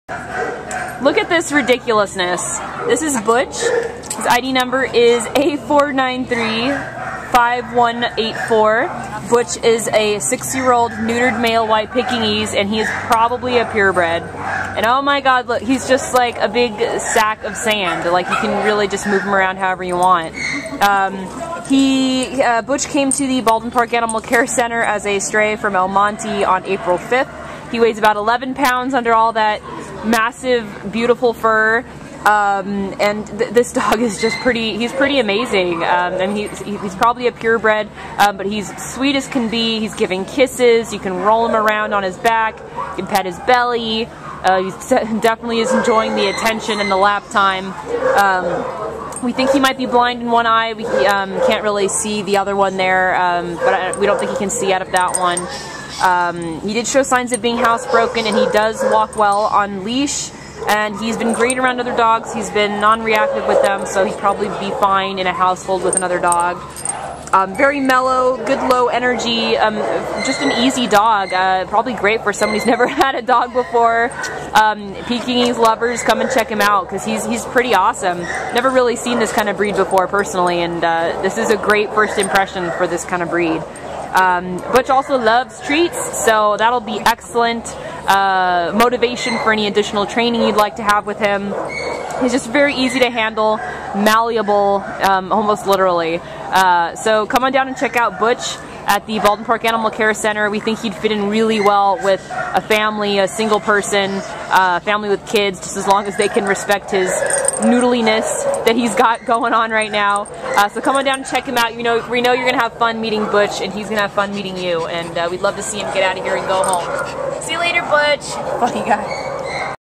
RESCUED! A4935184 Butch | Pekingese
A4935184 Butch is a spunky 6-year-old white neutered male Pekingese who was found as a stray in El Monte on April 5 and brought to the Baldwin Park Animal Care Center. Weighing 11 lbs, Butch shuffles along politely on leash, wagging his fluffy tail as he goes. He meets new people with good cheer and is also friendly toward other dogs. Butch has quite a pronounced silly streak: He loves belly rubs and responds so enthusiastically to getting his tummy touched that he actually wiggles across the floor. How did this delightful ball of fluff end up with such a tough name? For reasons unknown, Butch is missing one eye, giving him a rugged look. Butch is totally lovable, and will be a fantastic indoor best pal in any home with a sense of humor to appreciate this funny guy.